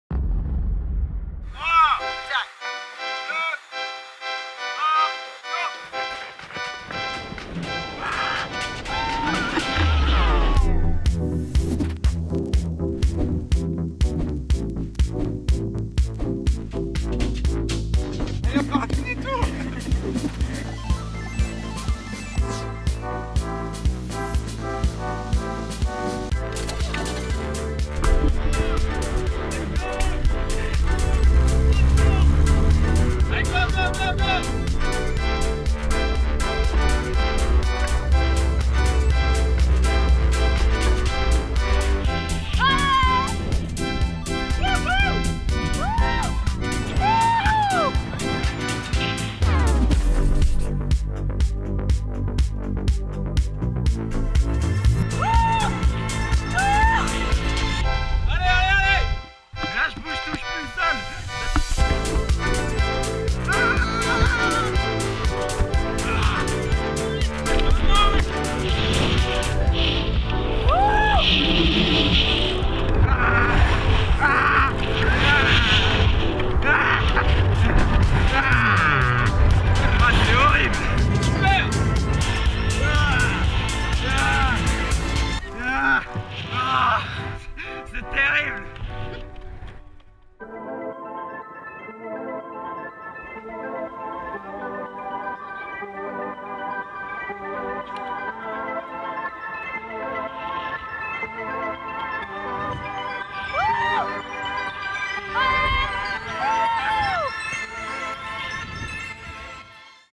GoPro Awards: Crazy Human Centrifuge
A few French students decided to take their homework to the next level. To start they built a simple structure, made of wood over the course of three days. A rope went from one side of the upper bar to the other provide enough length to attach with a harness. Then they dug a meter deep hole in the ground and put a PVC tube with rolling in it. A smaller structure was placed for the rope to wrap around which was then fixed on the mast. Then the two pulleys are placed to lead the rope and control the flow. They call it the "Centrifugal".